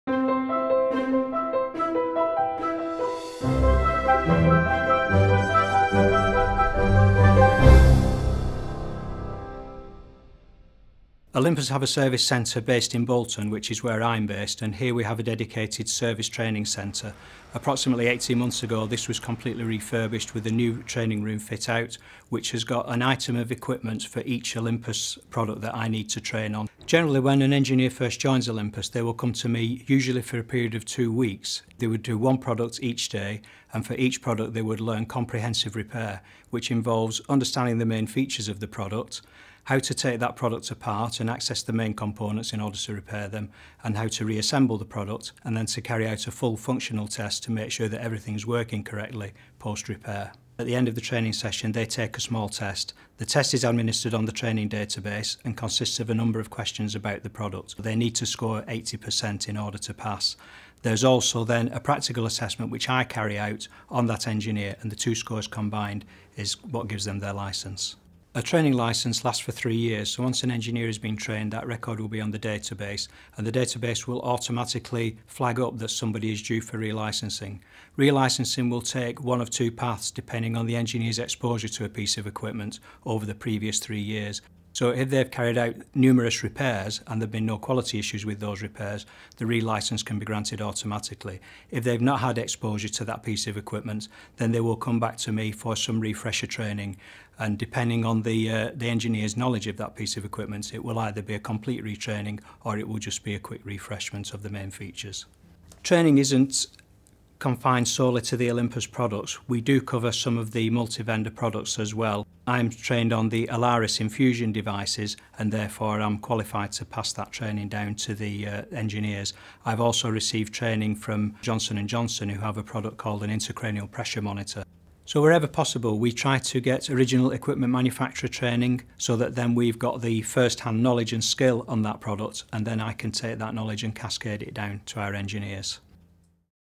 Olympus Medical Multi-Vendor Service (MVS) Engineer Training
Watch our video to learn about how our engineers are trained on Olympus and multi-vendor equipment as well as how we monitor engineer training licences and their competency